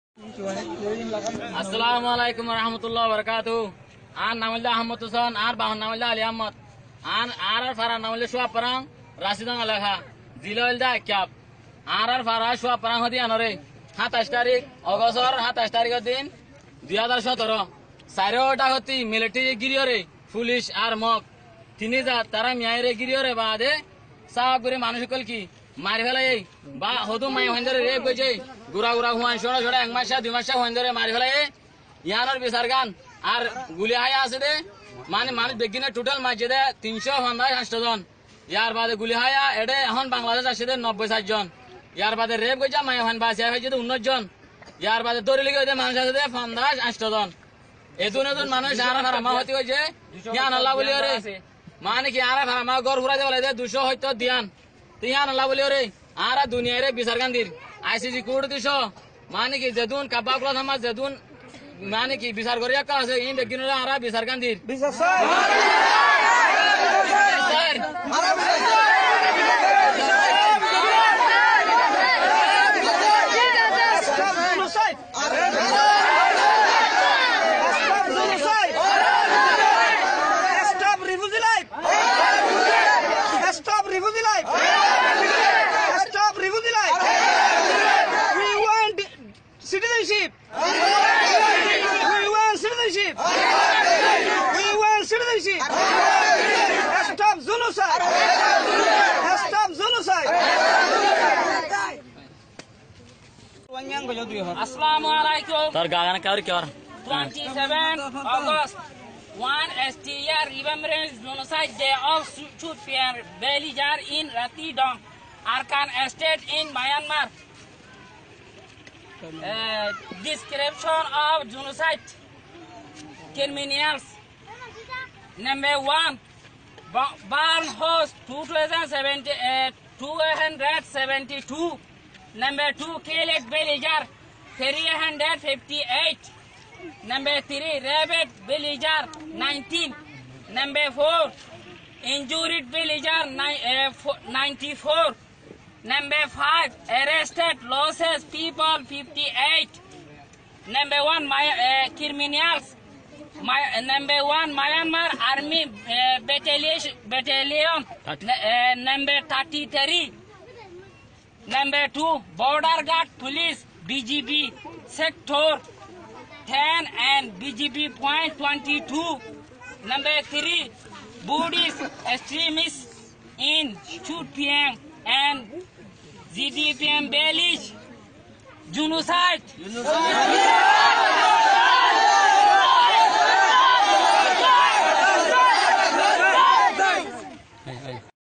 Chut Pyin villagers arguing for Justice
Chut Pyin Rohingya village of Rathedaung was surrounded by Myanmar Army, BGP and local Rakhine Buddhists on 27 August 2017.
They killed men, women and Children, raped and gang raped women, entire village has been burned.
Killed total: 358 including children & women
Raped/gang raped survivors: 19
Injured bullet hits: 94
Burned houses: 272
Arrested: 58 now in jails of Myanmar
The rest of the villages are escaped for life to Bangladesh Rohingya Refugee Camps.
Victims and witnesses are now living in the Rohingya Refugee Camps.